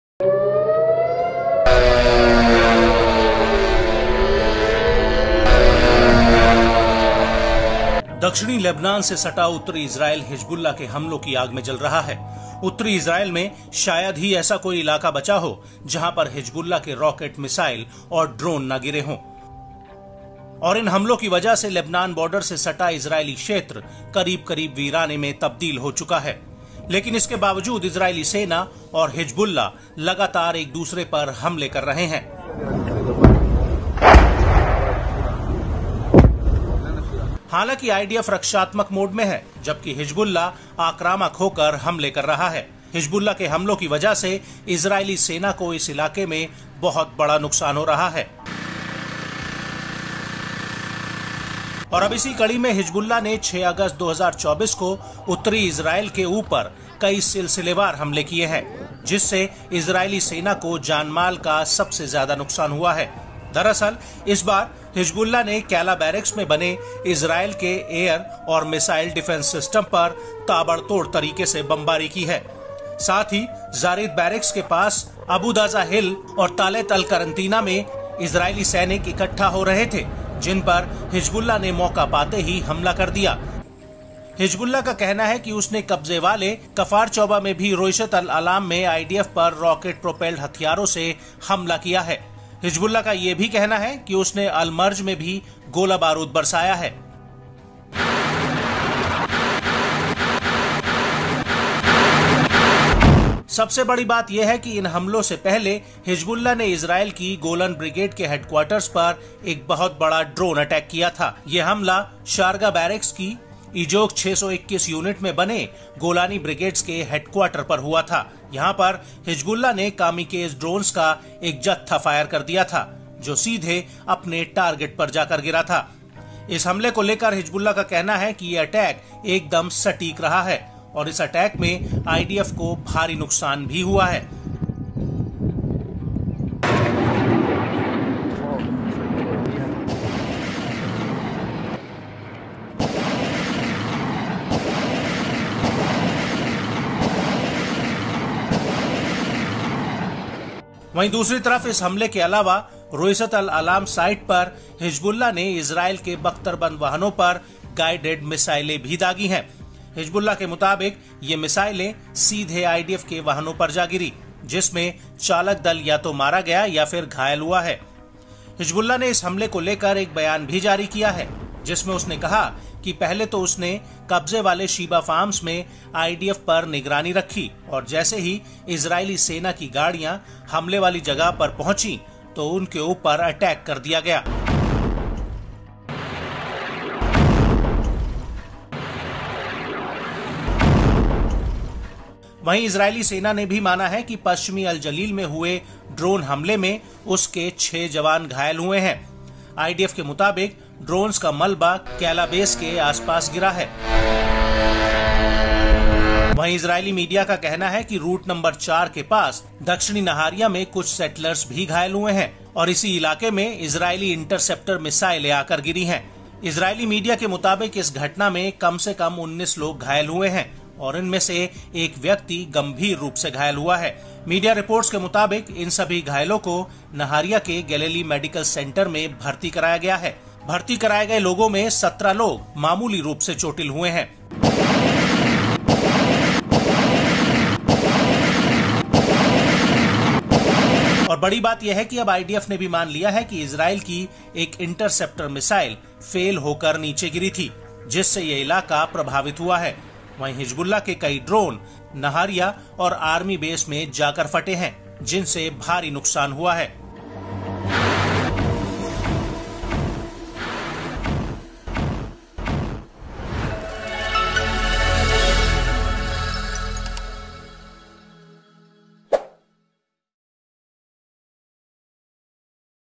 Israel Hamas War: इज़रायल पर सबसे बड़ा ड्रोन अटैक, कभी न पूरा होने वाला हो रहा नुकसान | N18G
Amidst the Israel Hamas War, Hezollah carried out a series of attacks on Northern Israel on 6 August 2024, causing the greatest loss of life and property to the Israeli Army. In fact, this time Hezbollah has bombed the Israel Air Defense System built in Kela Barracks in a rapid manner. Also, Israeli soldiers were gathering in Abu Dajaj Hill and Tallet Al-Qarantina near Zar'it Barracks, whom Hezbollah attacked as soon as it got the opportunity. Hezbollah says that it has also attacked the IDF in Roueisat Al-Alam in the occupied Kfar Chouba with rocket propelled weapons. Hezbollah also says that it has rained heavy ammunition on Al-Marj. Before these attacks, Hezbollah had carried out a huge drone attack on the Israel Golani Brigade Headquarter. This attack took place on the headquarters of the Golani Brigade located in the Egoz 621 Unit of Sharaga Barracks. Here Hezbollah fired a batch of Kamikaze, which fell directly on its target. Regarding this attack, Hezbollah says that this attack was very accurate and IDF has also suffered heavy losses in this attack.

Israel Hamas War के बीच Hezollah ने 6 अगस्त 2024 को Northern Israel के ऊपर कई सिलसिलेवार हमले किए हैं, जिससे Isaeli Army को जानमाल का सबसे ज्यादा नुकसान हुआ है. दरअसल, इस बार Hezbollah ने Kela Barracks में बने Israel Air Defense System पर ताबड़तोड़ तरीके से बमबारी की है. साथ ही, Zar'it Barracks के पास Abu Dajaj Hill और Tallet Al-Qarantina में Israeli Soldiers इक्कट्ठा हो रहे थे, जिन पर Hezbollah ने मौका पाते ही हमला कर दिया. Hezbollah का कहना है कि उसने कब्ज़े वाले kfar Chouba में भी Roueisat Al-Alam में IDF पर रॉकेट प्रोपेल्ड हथियार से हमला किया है. Hezbollah का ये भी कहना है कि उसने Al-Marj पर भारी गोलाबारूद बरसाया है. इन हमलों से पहले Hezbollah ने Israel Golani Brigade Headquarter पर एक बहुत बड़ा ड्रोन अटैक किया था. यह हमला Sharaga Barracks की Egoz 621 Unit में बने गोलानी ब्रिगेड के हेडक्वॉर्टर पर हुआ था. यहां पर हिजबुल्लाह ने Kamikaze का एक जत्था फायर कर दिया था, जो सीधे अपने टारगेट पर जाकर गिरा था. इस हमले को लेकर हिजबुल्लाह का कहना है ये अटैक एकदम सटीक रहा है और इस अटैक में IDF को भारी नुकसान भी हुआ है.